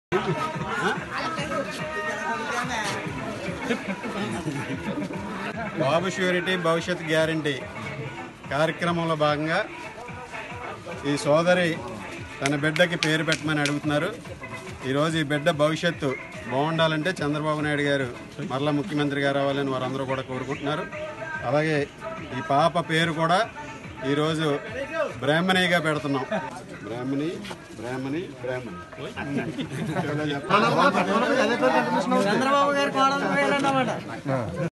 బ్రాహ్మణి... బ్రాహ్మణి.. బ్రాహ్మణి అంటూ పాపకు పేరు పెట్టి, మూడుసార్లు పిలిచారు ఆరిమిల్లి రాధాకృష్ణ.
"బ్రాహ్మణి... బ్రాహ్మణి... బ్రాహ్మణి" అంటూ పాపకు పేరు పెట్టి, మూడుసార్లు పిలిచారు టీడీపీ మాజీ ఎమ్మెల్యే ఆరిమిల్లి రాధాకృష్ణ. తణుకు నియోజకవర్గం, ఇరగవరం గ్రామం, గొల్లమాలపల్లి ఎస్సీ కాలనీలో టీడీపీ అభిమాని అయిన ఎస్సీ సోదరి కోరికపై ఆమె బిడ్డకు బ్రాహ్మణి అని రాధాకృష్ణ పేరు పెట్టారు